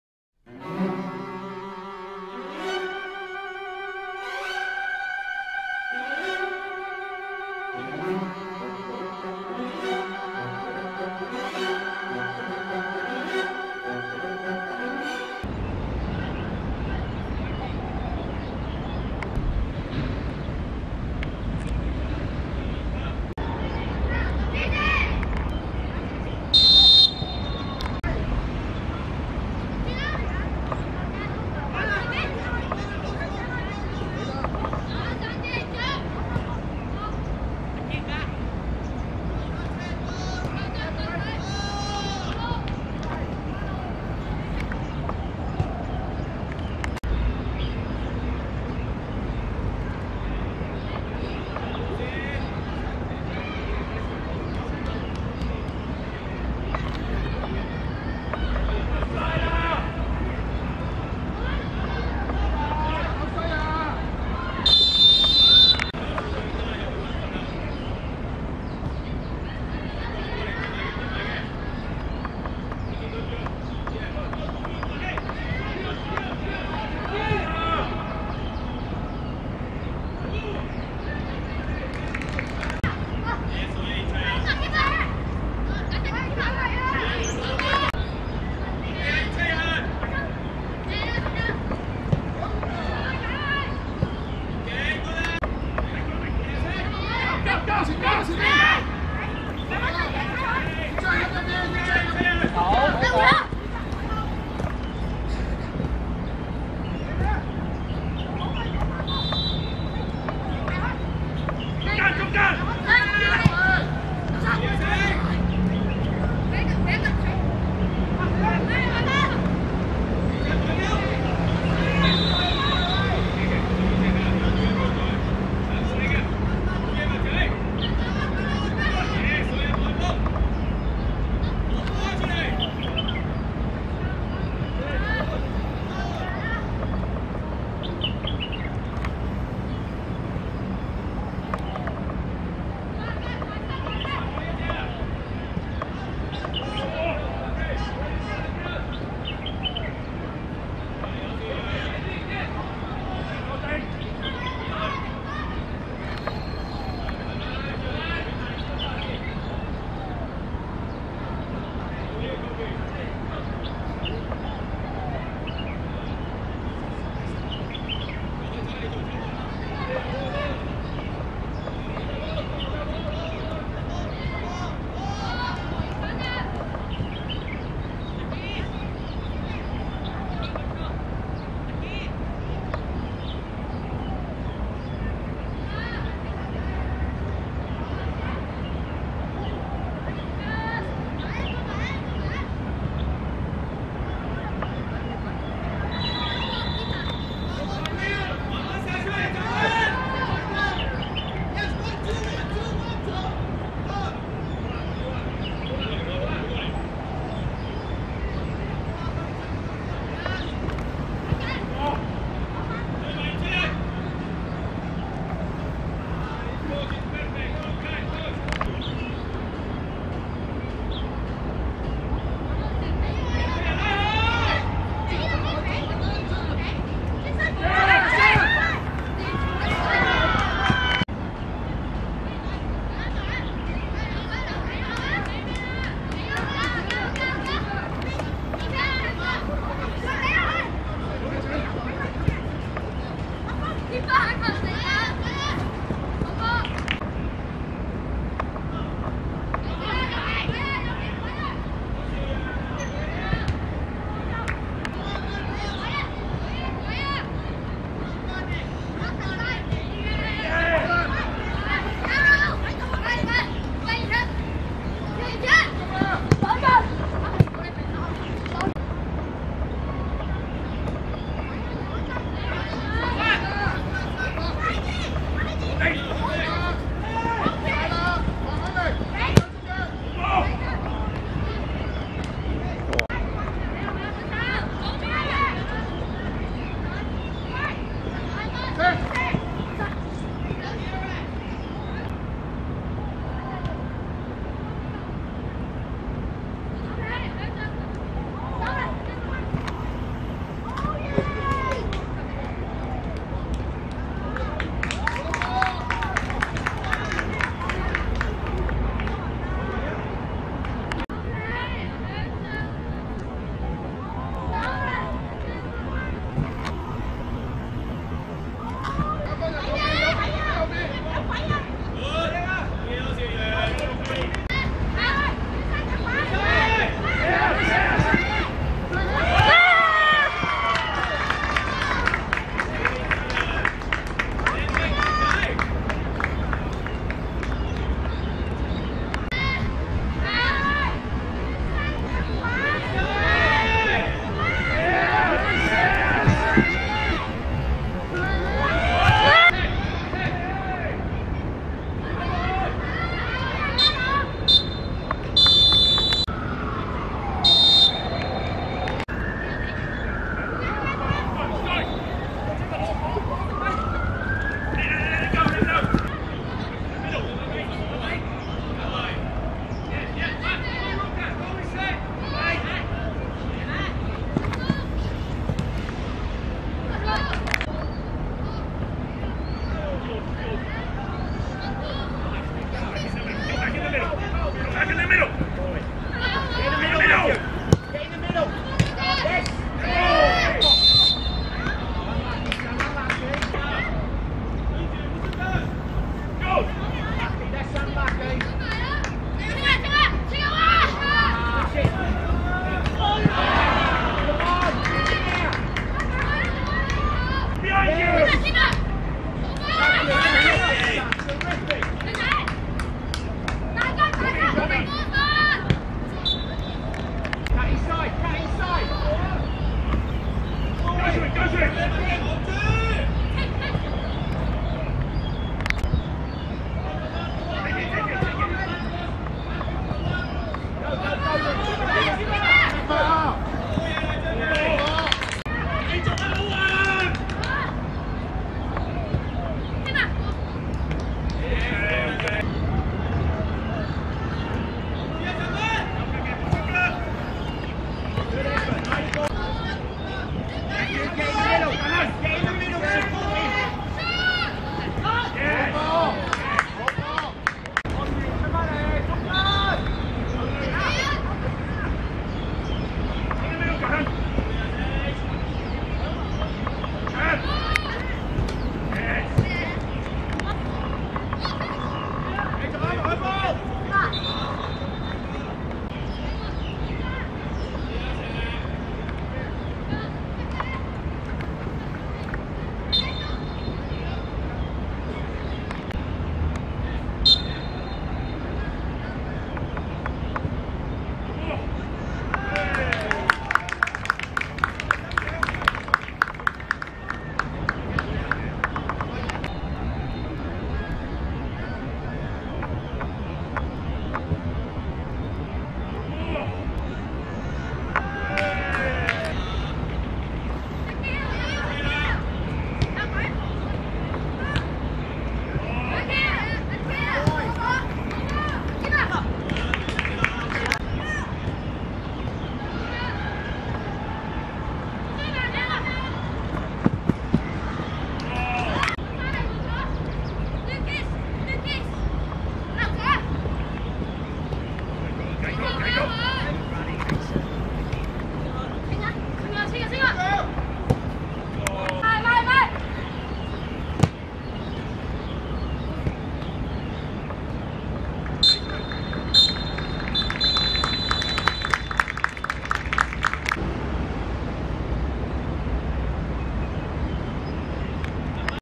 傑志vs元朗(2016.12.4.青少年足球U13甲組聯賽)精華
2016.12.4.賽馬會傑志訓練中心球塲 09:00 傑志(深藍) 0 : 3 元朗(淺藍)